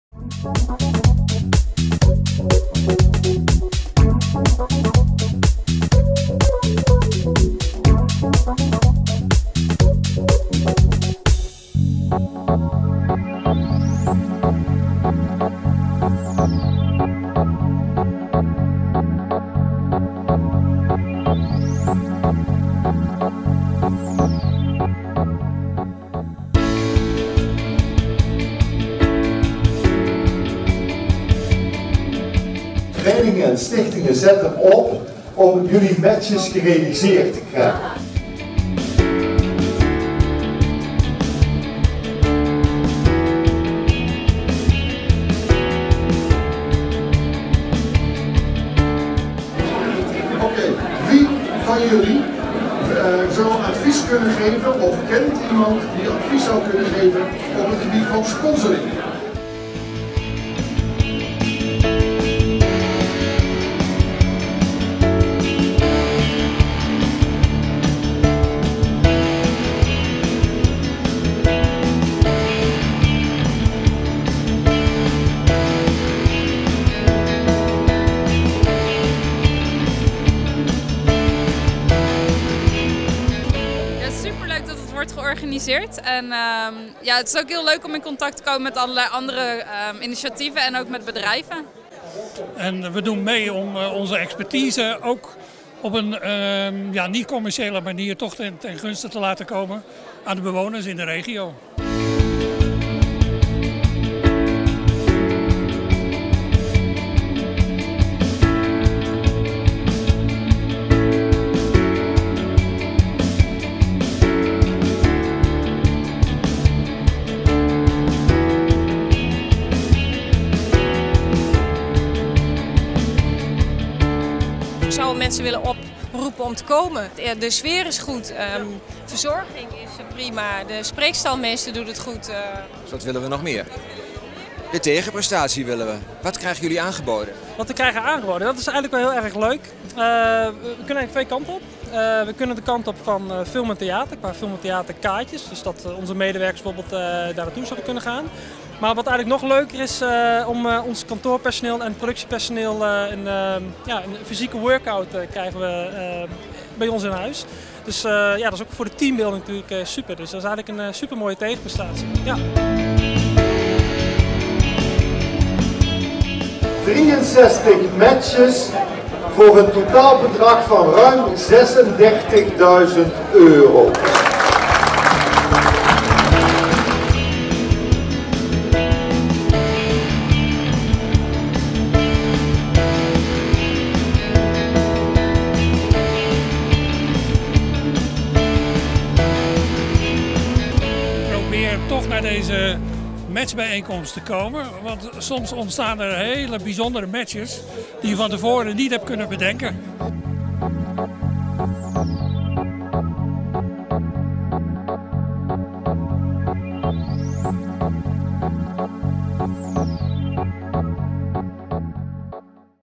Beursvloer de Liemers 2018.
Ook dit jaar is er weer een Beursvloer de Liemers. Op 19 november 2018. Verenigingen en stichtingen maken matches met bedrijven. Afspraken over de hulp en de tegenprestaties worden bekrachtigd door een notaris.